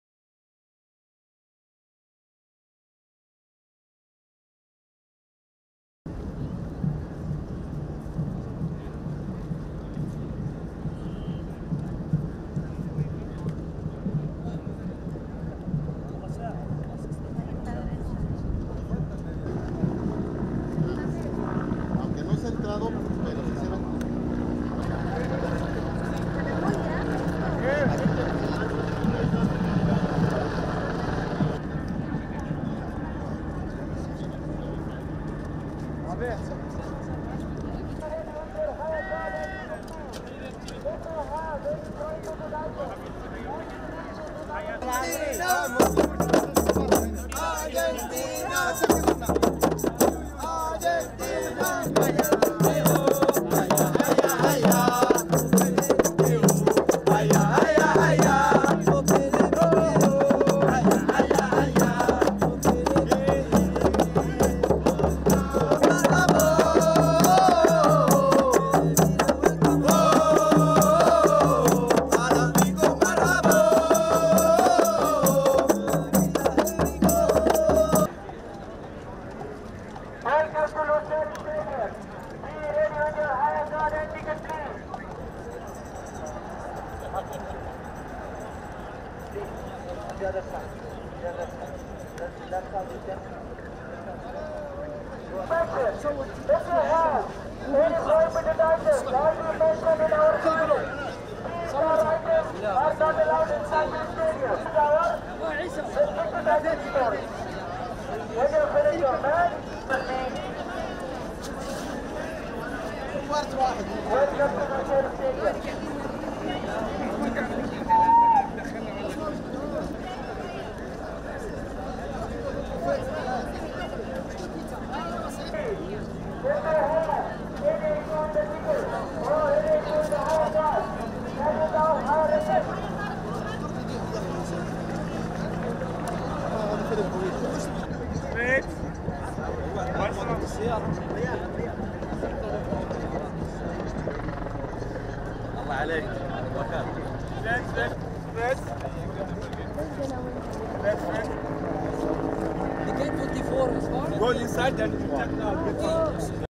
Suudi Arabistan taraftarından Arjantin maçında renkli görüntüler
GÖRÜNTÜ DÖKÜMÜ :

- Taraftarların Lusail Stadyumu önünde toplanması
- Taraftarların stadyuma girişi
- Atlı polis birliklerin bölgede güvenlik sağlaması DOHA (AA) - Katar'ın ev sahipliğini yaptığı 2022 FIFA Dünya Kupası’nda, C Grubu'nda yer alan Arjantin ile Suudi Arabistan takımları, Lusail Stadyumu'nda karşılaştı. Mücadele öncesi taraftarlar stadyum önünde toplandı.